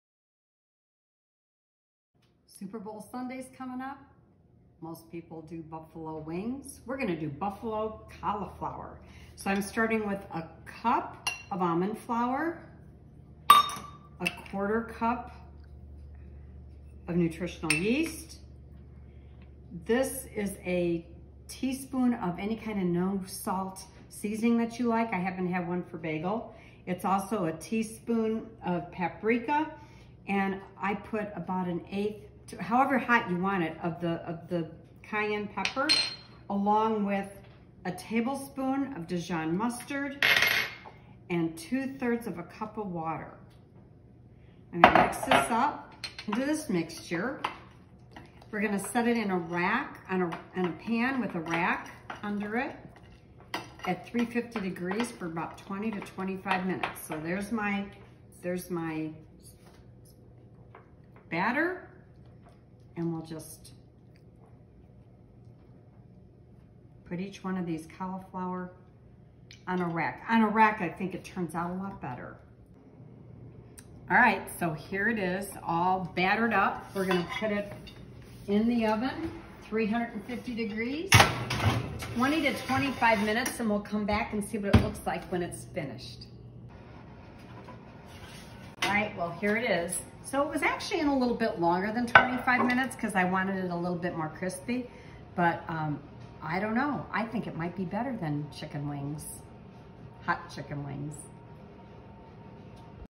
Buffalo Cauliflower SD 480p
INGREDIENTS:

1 cup Almond Flour
1/4 Cup Nutritional Yeast
1 tsp “no salt” seasoning blend
1tsp Paprika
1/4-1/2 tsp cayenne pepper ( or to taste)
2/3 cup water
1 Tbs Dijon mustard
1 head cauliflower, cut into florets

Instructions:

Combine flour, nutritional yeast and seasonings in a bowl. Using a whisk add water ,
add water,stir in mustard.  Mixture should resemble a thick batter

Toss cauliflower florets with the coating.  Place on a parchment-lined baking sheet or
on a baking rack.  Bake 25-35 minutes or until coating is dry and cauliflower is tender.